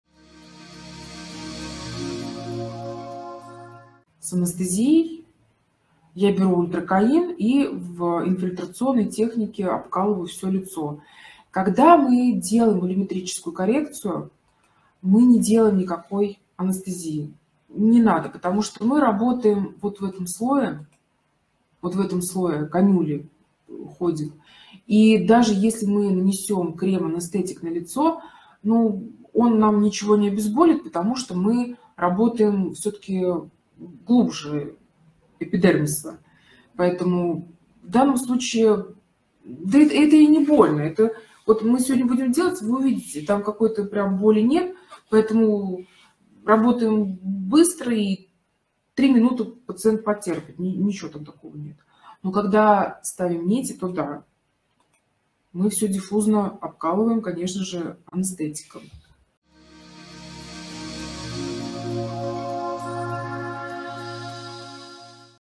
Какая анестезия предпочтительна?
Подготовка нижней трети лица к нитевому лифтингу: какая анестезия предпочтительна?
Об этом рассказывает Вопилкина Елена Валерьевна, хирург, врач-косметолог, эксперт Corneal.